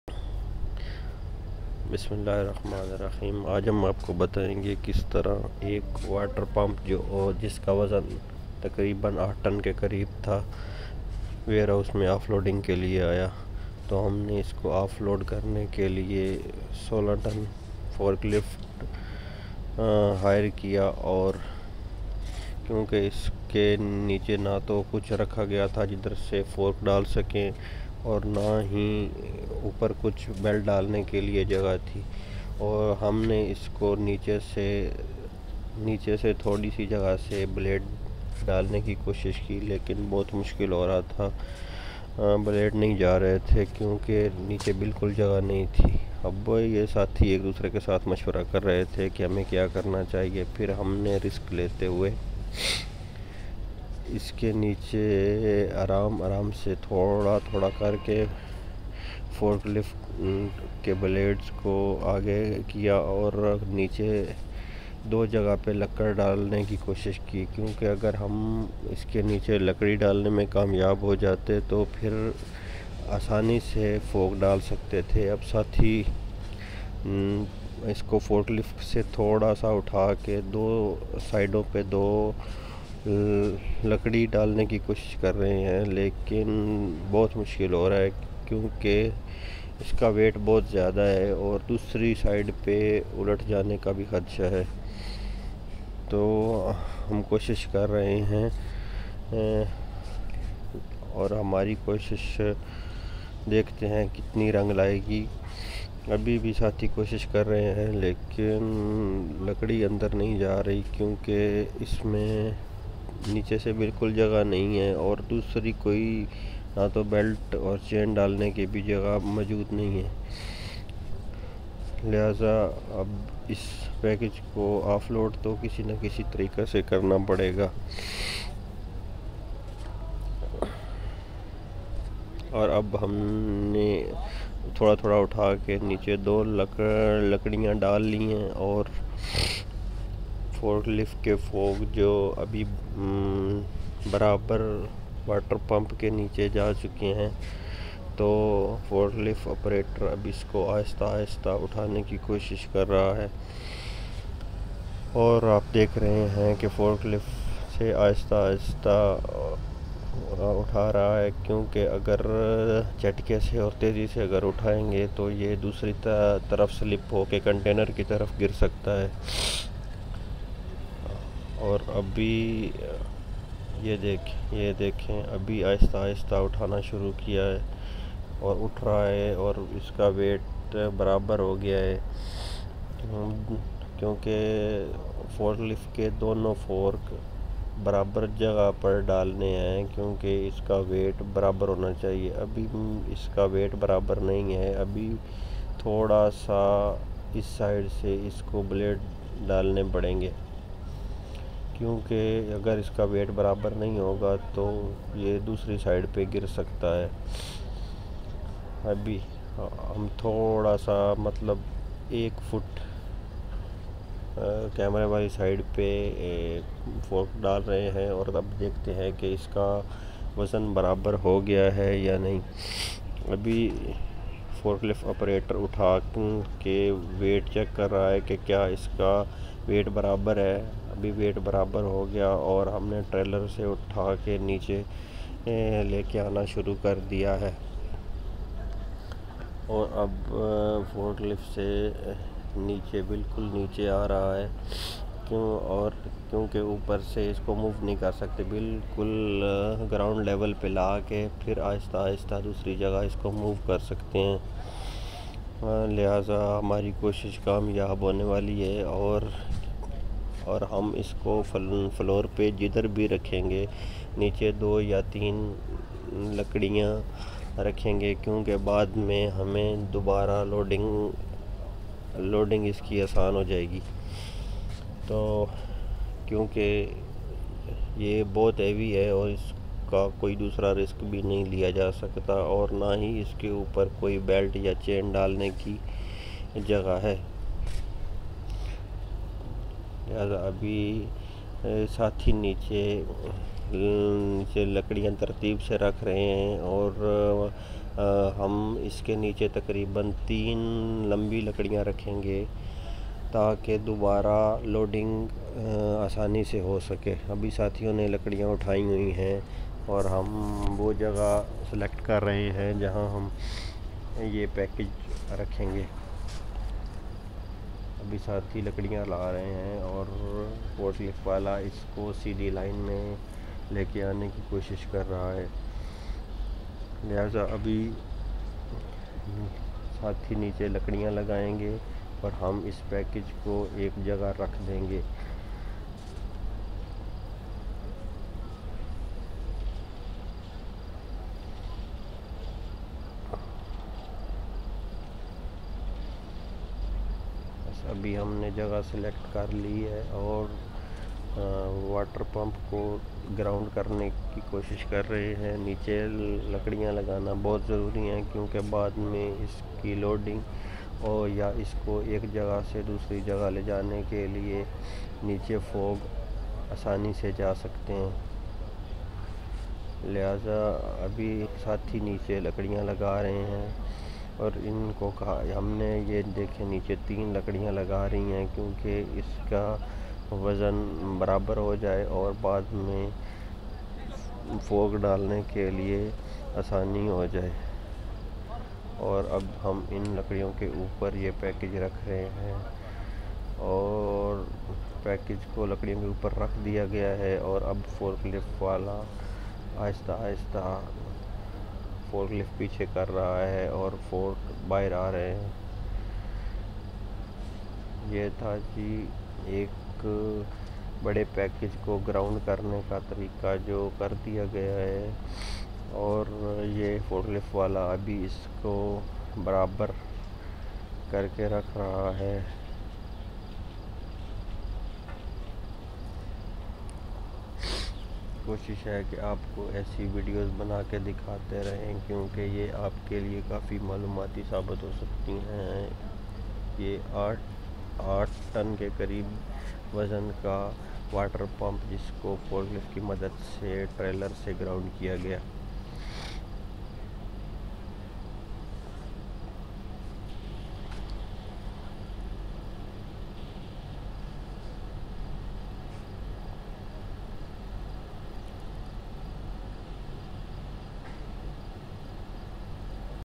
Very Heavy water pump weight 8 tons Offload without crane.
how to offload 8 tons water pump with Forklift